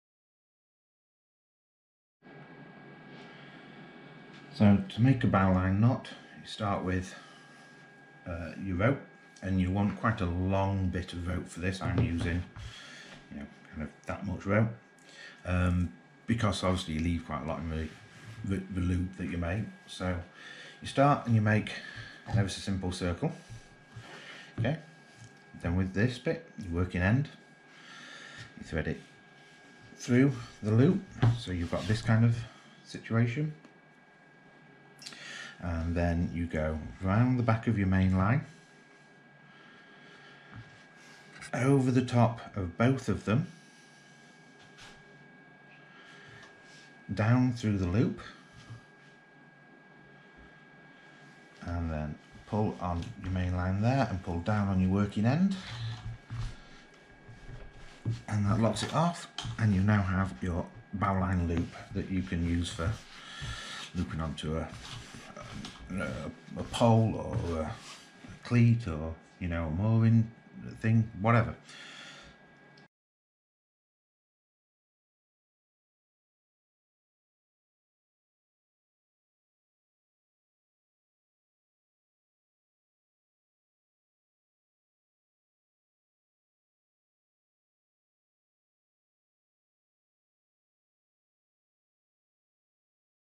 How to tie a Bowline Knot
Quick tutorial on How to tie a Bowline Knot this is a very useful not for boating but also many other situations